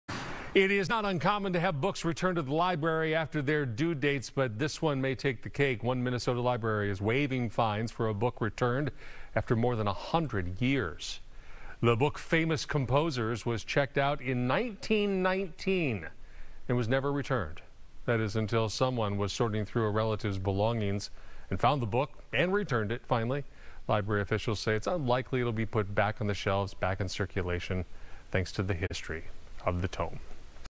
Book returned to library after more than 100 years
One book in Minnesota was returned to a library and no late fees will be charged.